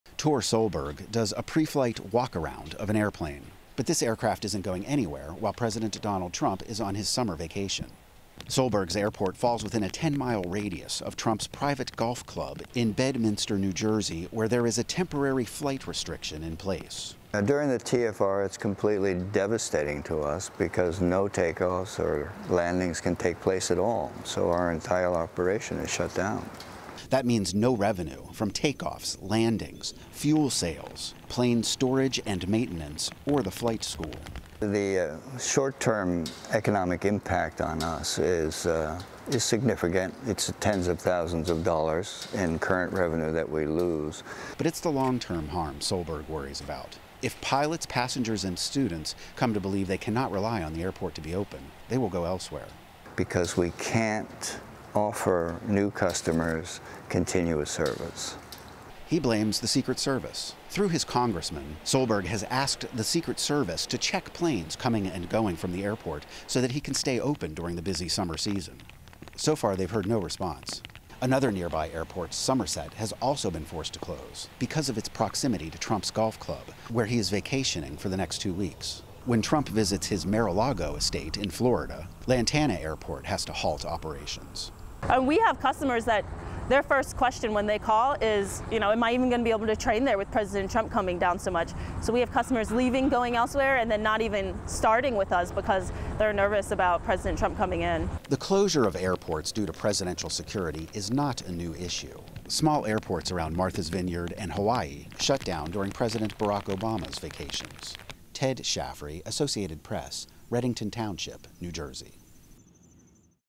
Trump Vacations Halt Local Airport Business
It’s high season for small airports that cater to amateur fliers and business clients, except if they’re located near President Donald Trump’s golf course in central New Jersey. Those airports are forced to halt operations. (August 8)